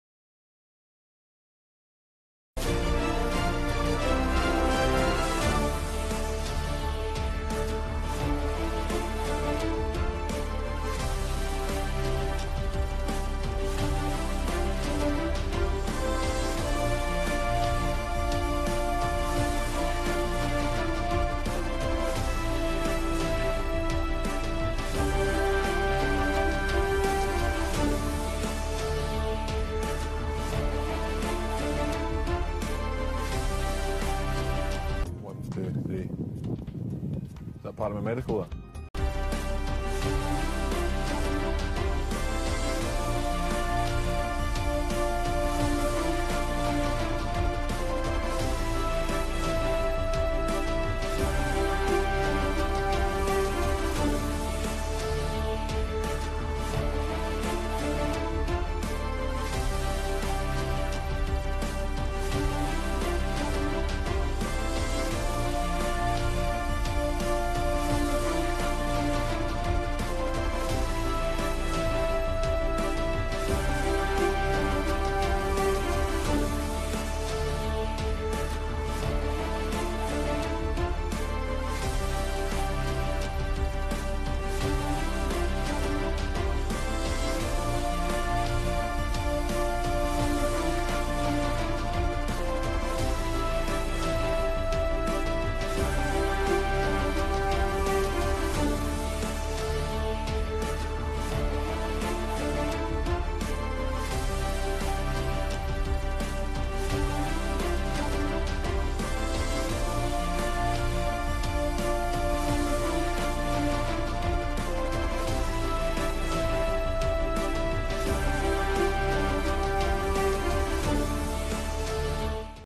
All of Rafa Benitez's signings on the day they signed
Every first team player signed by Rafa Benitez over the Summer is put together in this video on the day they signed, courtesy of NUFC TV.